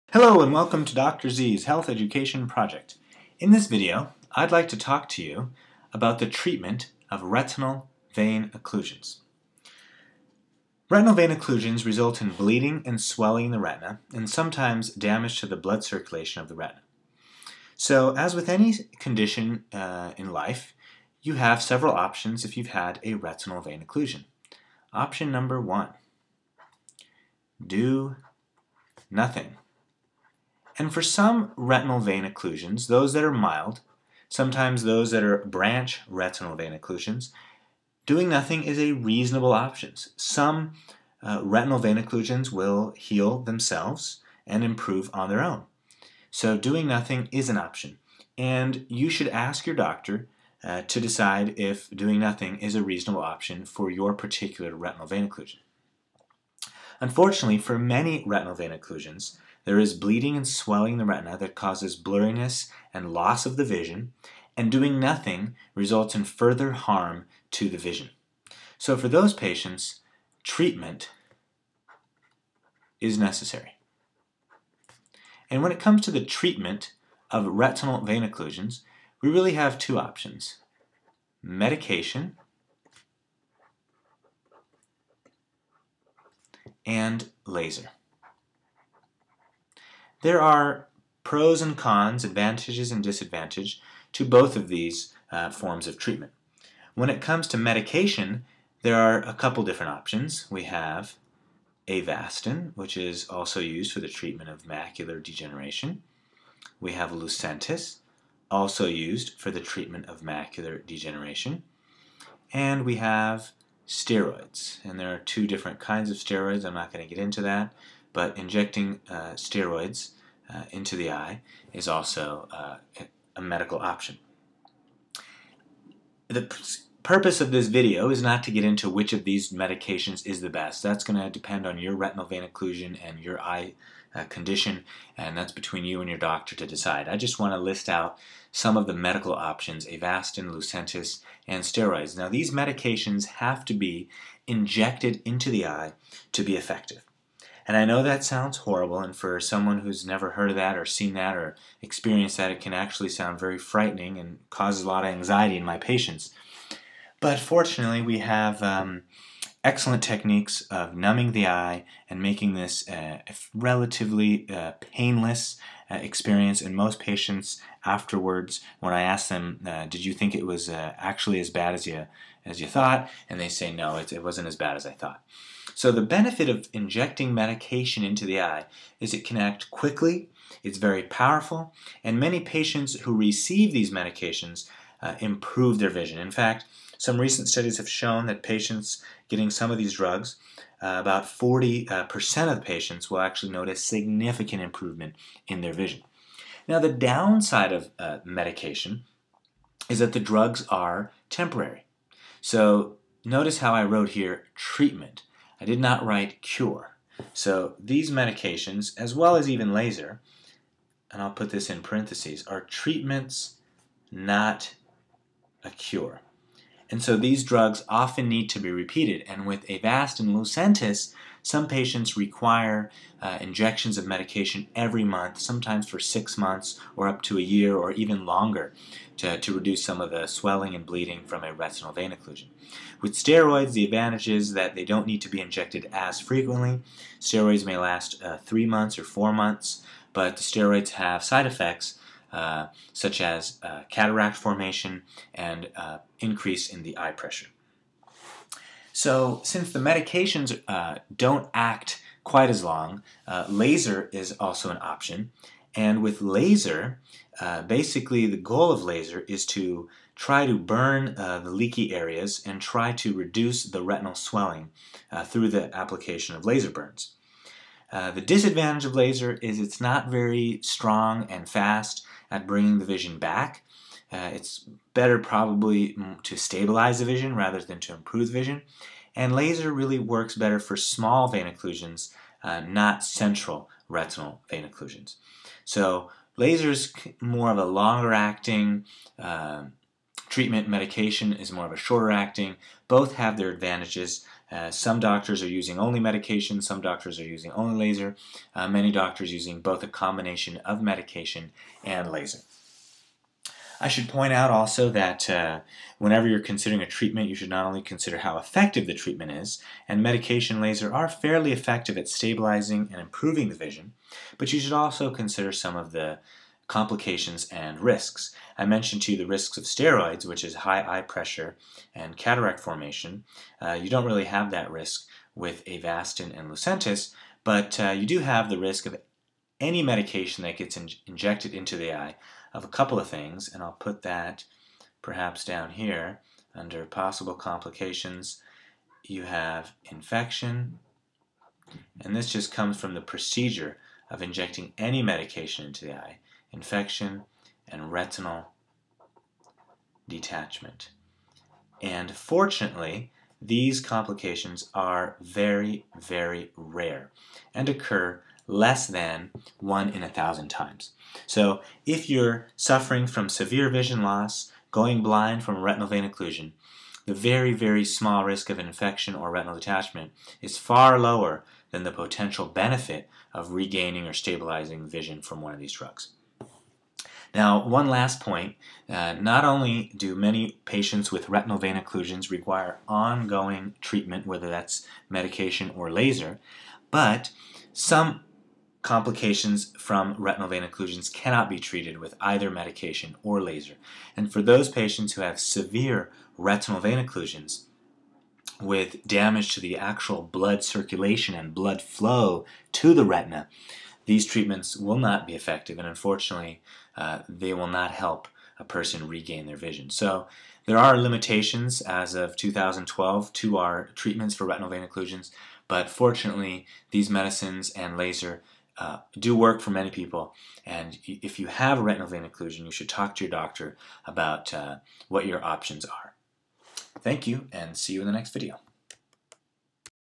Retinal vein occlusion treatment
Welcome to Dr. Z's health education project.  In this video I talk about the treatment options for a retinal vein occlusion.  This video does not substitute for consultation with your physician.